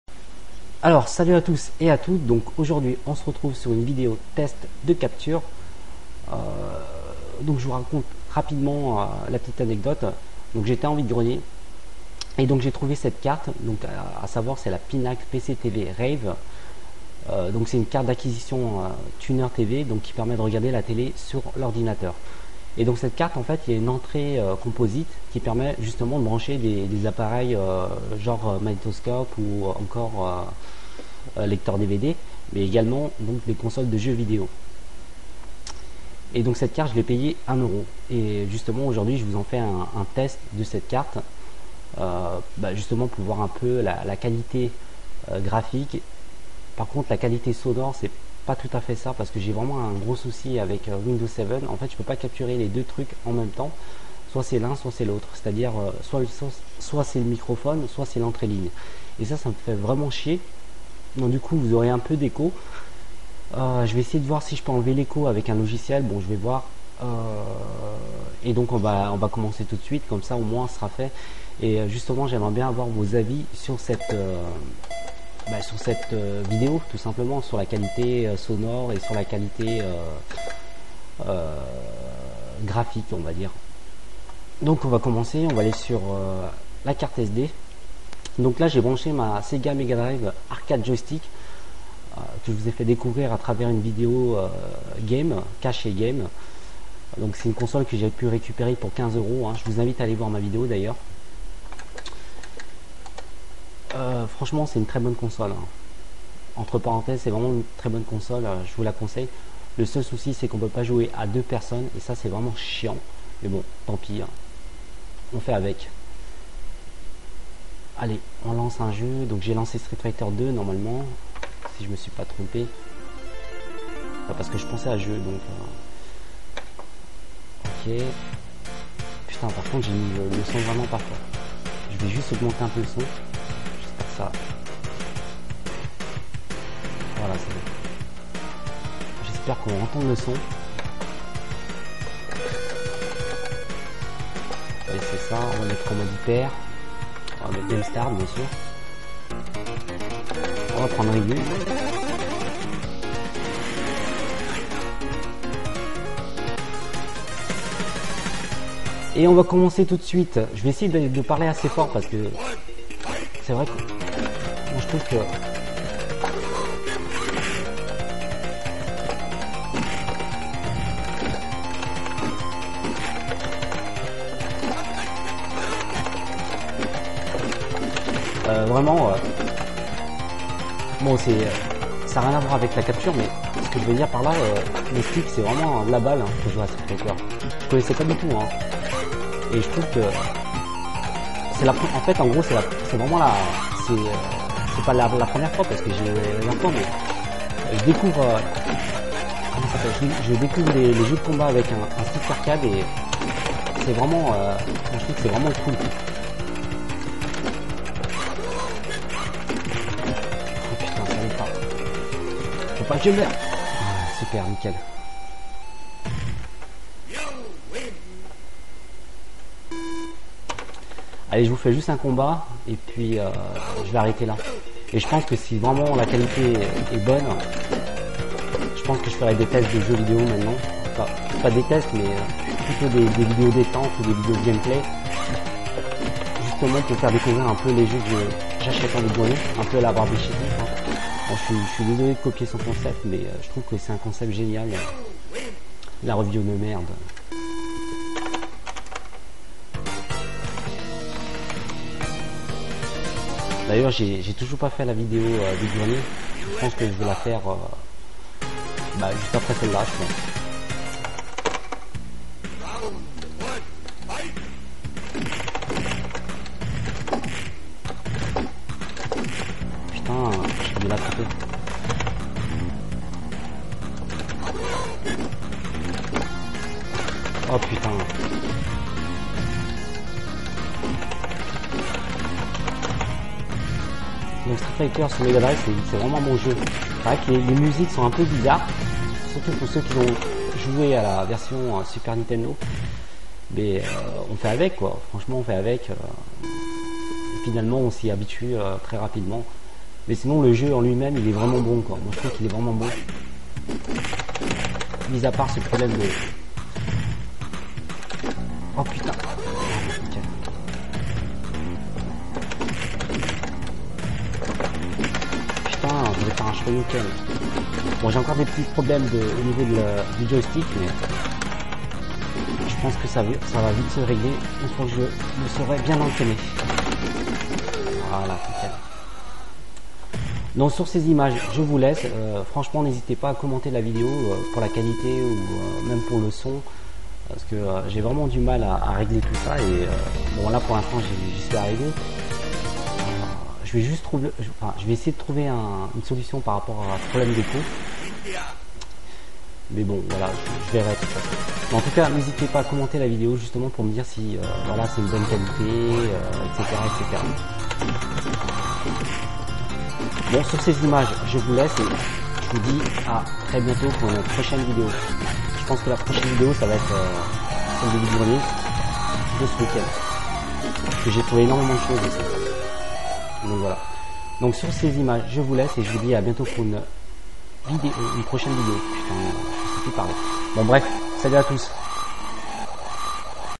Test Capture Pinnacle Pc TV Rave" HB Arcade Joystick Sega Megadrive"
Une très bonne alternative à un Dazzle, et pour pas chère!